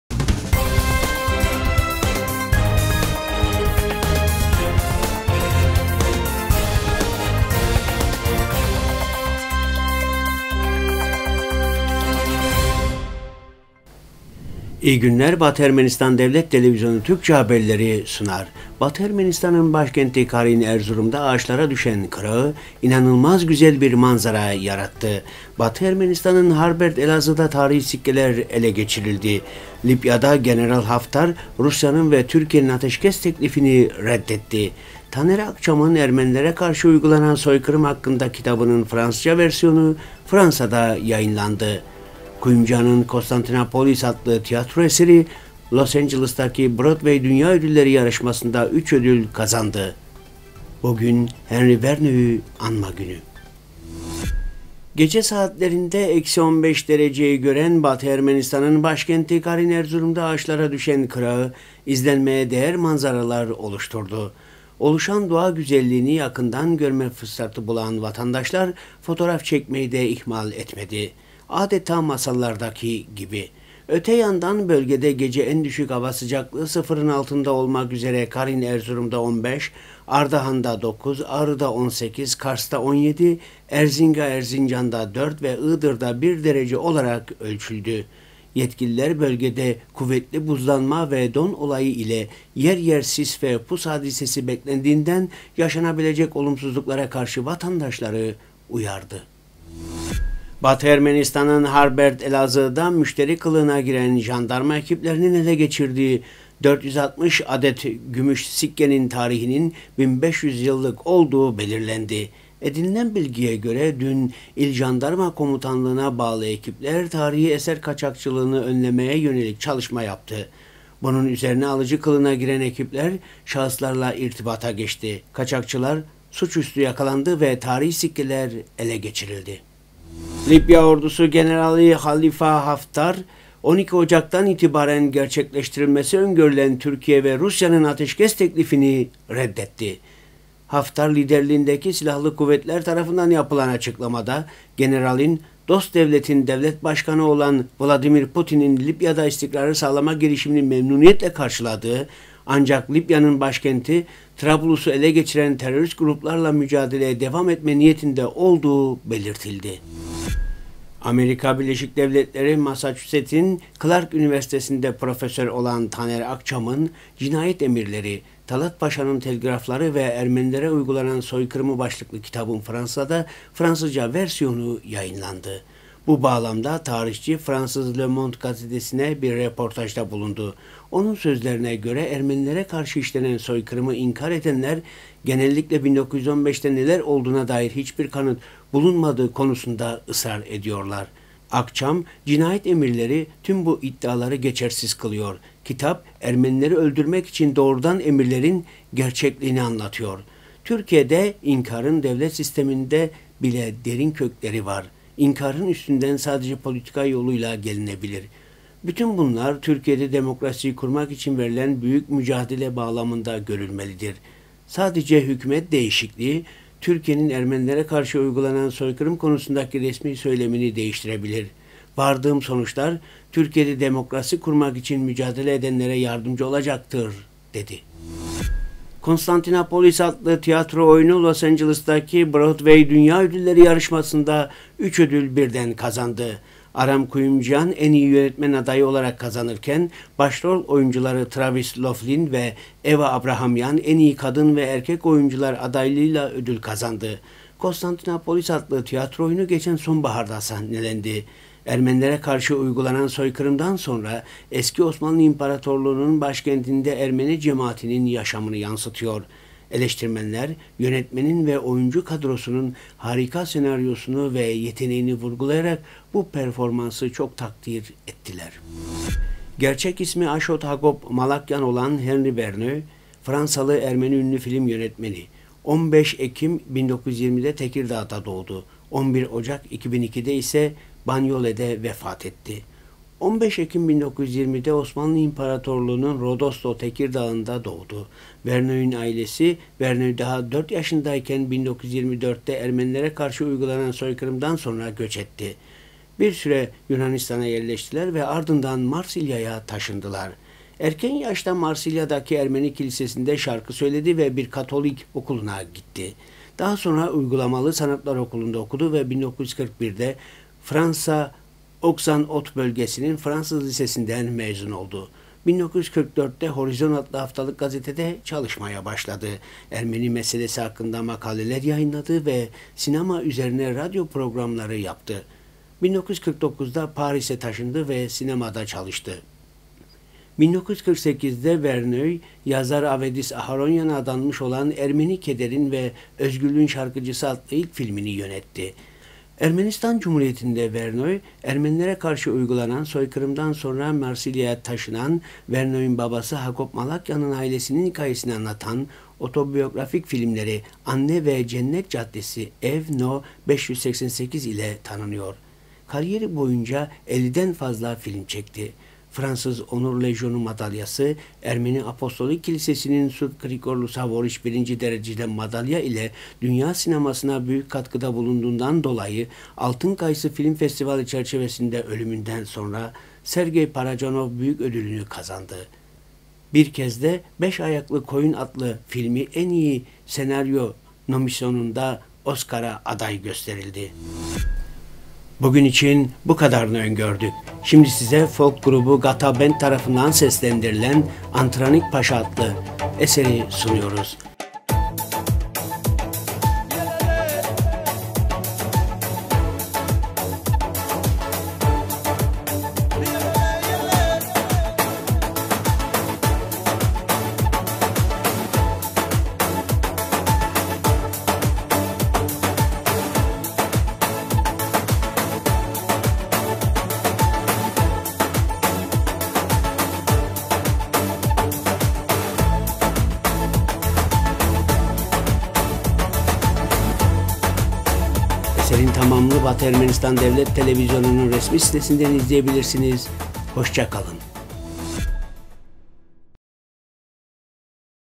Libya'da General Haftar Rusya'nın ve Türkiye'nin ateşkes teklifini reddetti․Haberler 11-01-2020
1- Batı Ermenistan’ın başkenti Karin-Erzurum'da ağaçlara düşen kırağı inanılmaz güzel bir manzara yarattı
2- Batı Ermenistan’ın Kharbert- Elazığ'da tarihi sikkeler ele geçirildi
3- Libya'da General Haftar Rusya'nın ve Türkiye'nin ateşkes teklifini reddetti
4- Taner Akçam'ın Ermenilere karşı uygulanan soykırım hakkında kitabının Fransızca versiyonu Fransa'da yayınlandı
5- Kuyumjyan'ın "Konstantinopolis" tiyatro eseri Los Angeles'deki Broadway Dünya Ödülleri yarışmasında 3 ödül kazandı
6- Bugün Henry Vernoy’ü Anma Günü